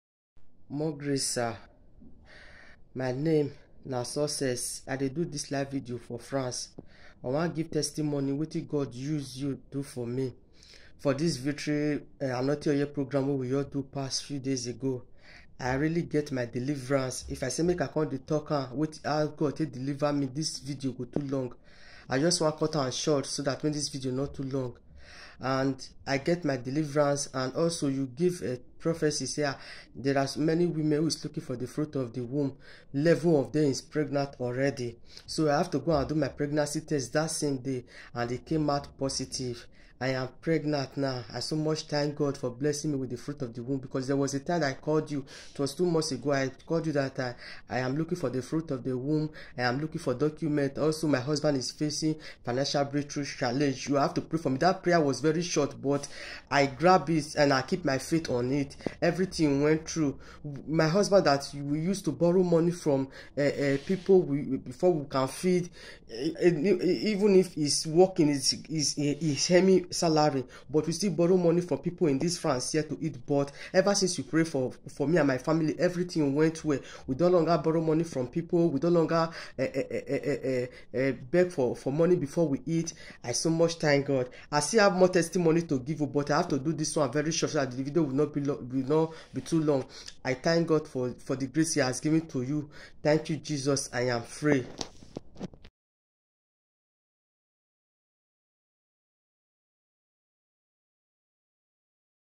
WATCH HOW THIS WOMAN RECEIVED HER INSTANT TESTIMONY THROUGH VICTORY ANOINTED OIL
More Grace TV Is a Christian Broadcasting Satellite . Bringing God's Grace To Your World